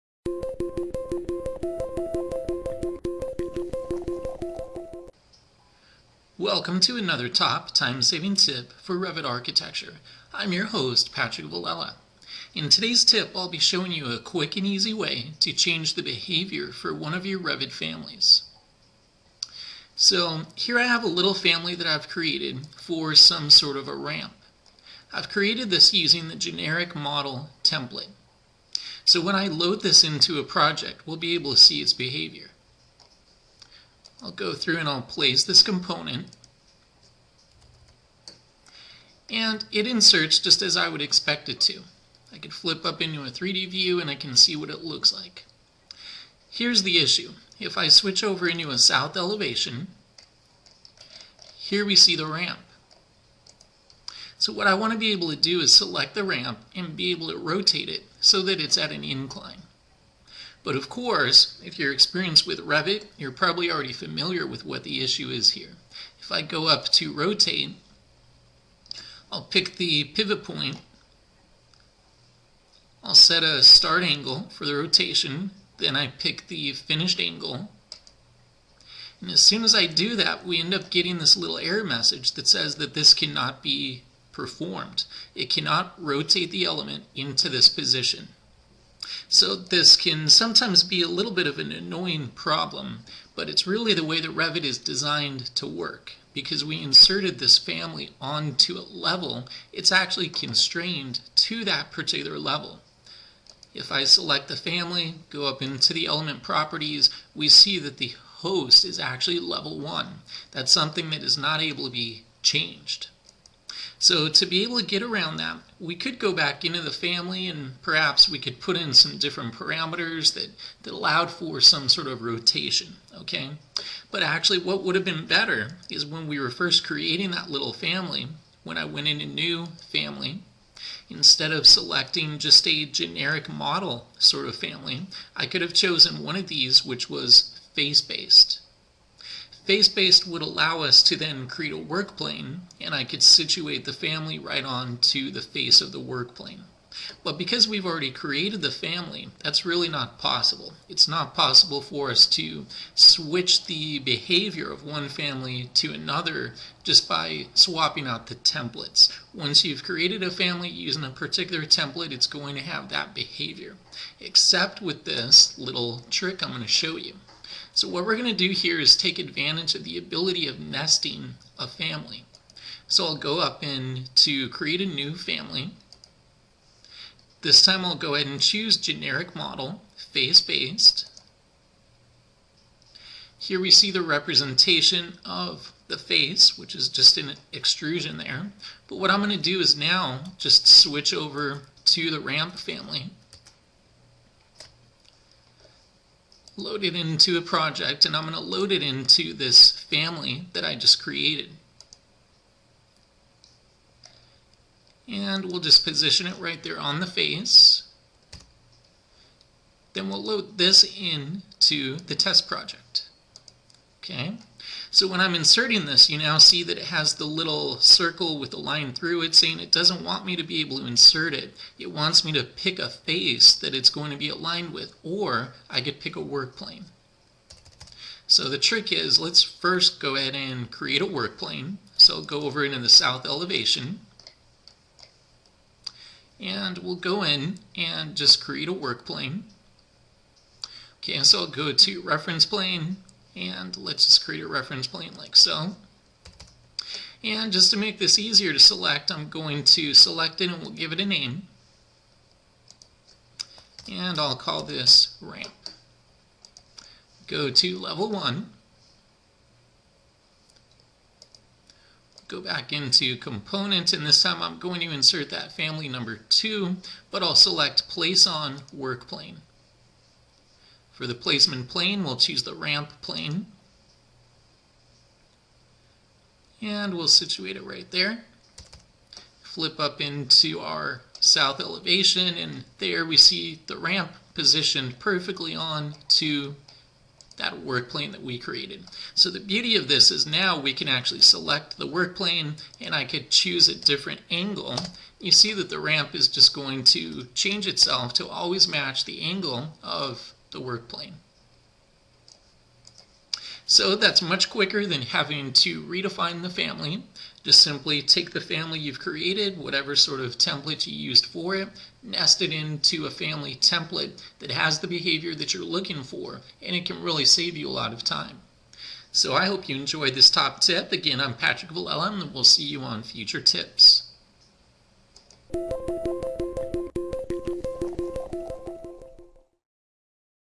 Revit Training - Changing Family Behavior
Have you ever created a Revit family using the wrong family template? Or maybe you created a wall hosted family when you really want it to be a ceiling based family? You don't need to start all over. It is possible to change your family's behavior. Check out this very useful tip to modify your Revit family.